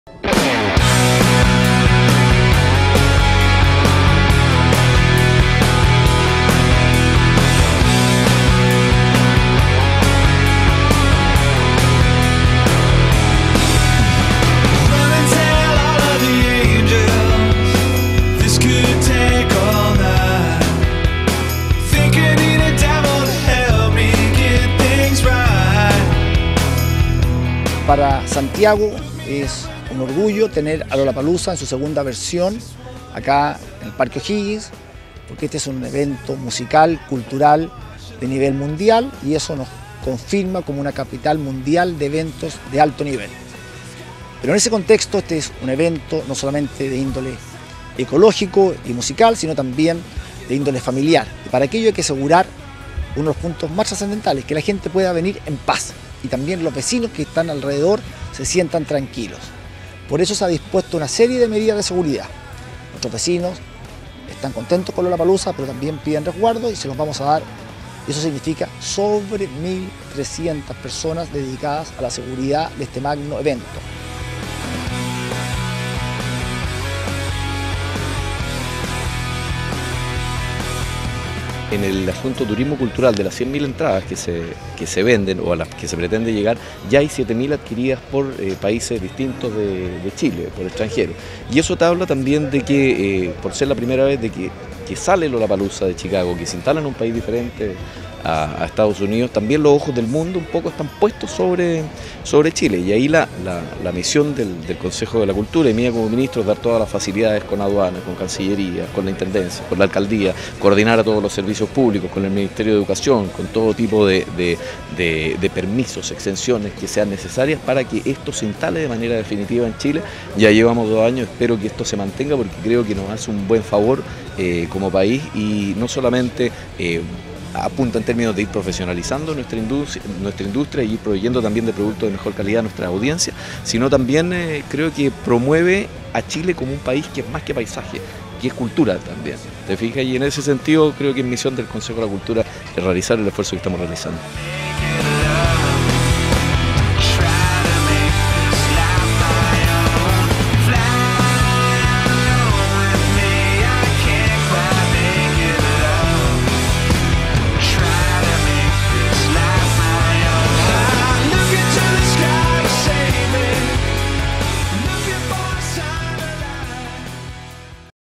Autoridades y organizadores ultiman detalles para Lollapalooza Chile
El alcalde de Santiago, Pablo Zalaquett y el ministro de Cultura, Luciano Cruz-Coke, contaron cómo se ultiman los detalles para realizar Lollapalooza Chile este sábado y domingo. (Video: Francisco González)